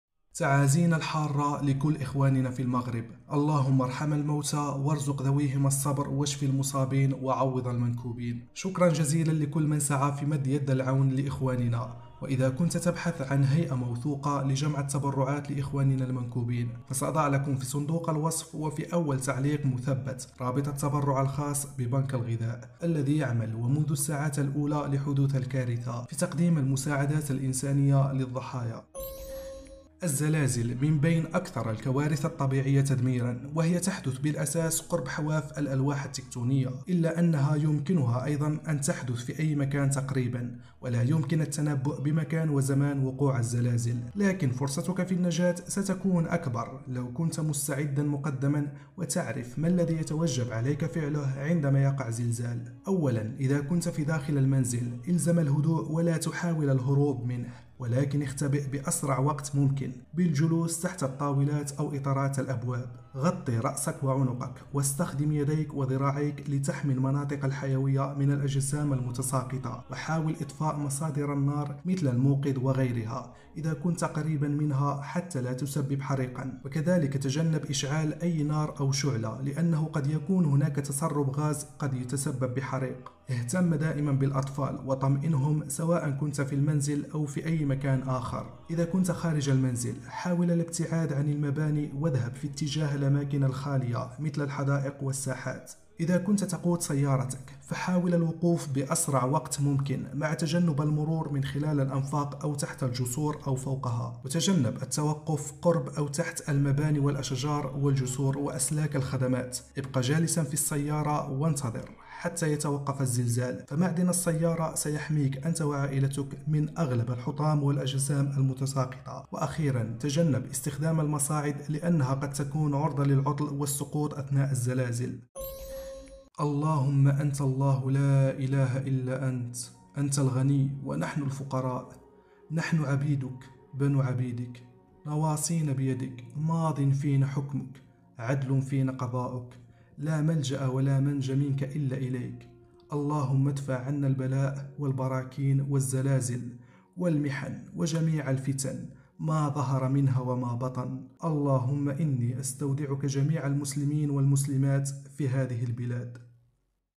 كيفية النجاة من زلزال أرضي! | ساهم في حملة جمع التبرعات لضحايا زلزال المغرب
معلومات قد تنقذ حياتك يوماً ما.. الزلازل من بين أكثر الكوارث الطبيعية تدميرًا وهي تحدث أساسا قرب حواف الألواح التِكتونية، إلا أنها يمكنها أيضًا أن تحدث في أي مكان آخر تقريبًا، ولا يمكن لأي وسيلة علمية التنبؤ بمكان وزمان حدوث الزلازل، لكن تبقى فرصتك في النجاة أكبر لو كنت مستعدًا مقدمًا وتعرف ما الذي يتوجب عليك فعله عندما يقع زلزال، وأنت بداخل المنزل أو خارجه